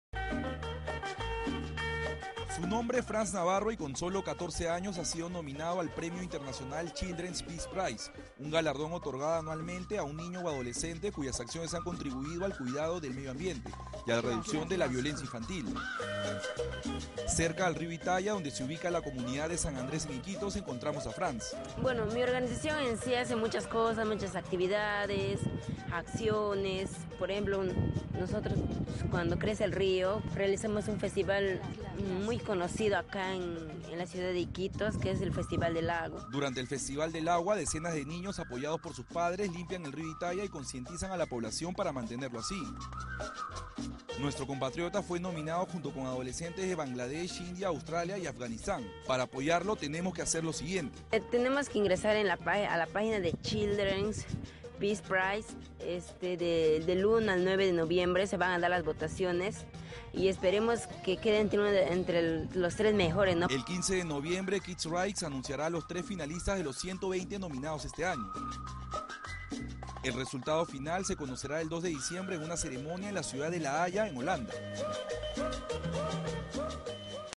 Iquitos: peruano de 13 años fue nominado a premio internacional
Nuestro compatriota fue nominado junto con adolescentes de Bangladesh, India, Australia y Afganistán al premio “Children´s Peace Prize”.